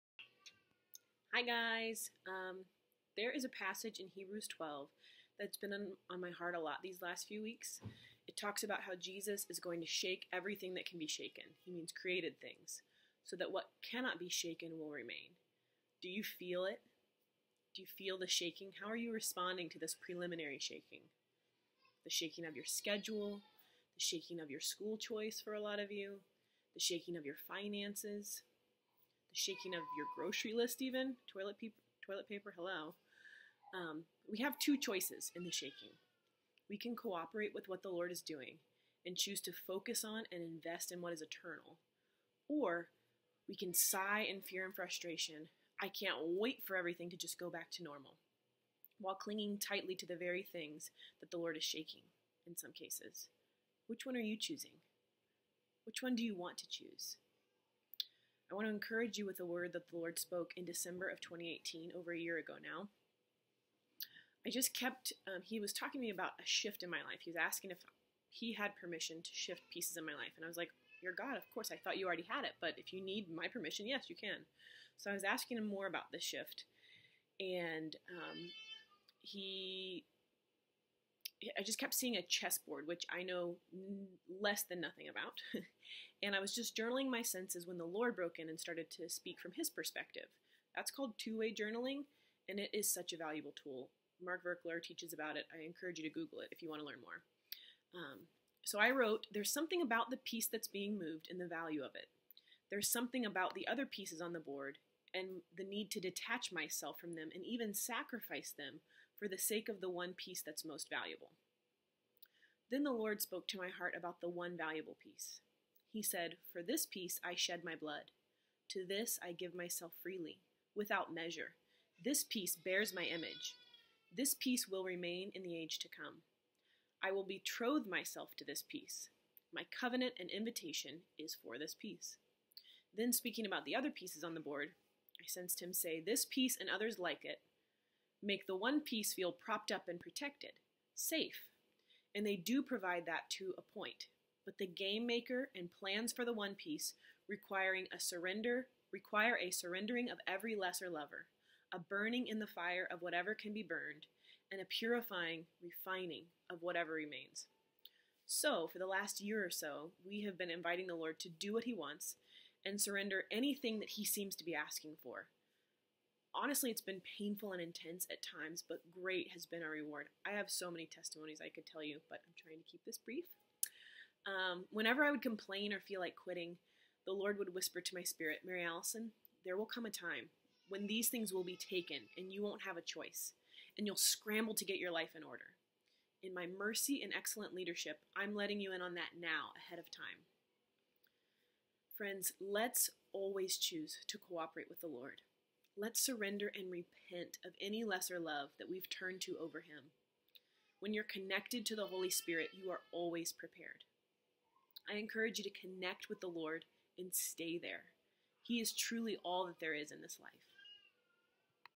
Responding in Times of Shaking - Dayton SHOP
Dayton SHOP is located in Dayton, OH. In this video, Mary Allison Toppins references Hebrews 12:25-29.

See to it that you do not refuse him who speaks. If they did not escape when they refused him who warned them on earth, how much less will we, if we turn away from him who warns us from heaven? At that time his voice shook the earth, but now he has promised, “Once more I will shake not only the earth but also the heavens.” The words “once more” indicate the removing of what can be shaken—that is, created things—so that what cannot be shaken may remain.

Therefore, since we are receiving a kingdom that cannot be shaken, let us be thankful, and so worship God acceptably with reverence and awe, for our “God is a consuming fire.”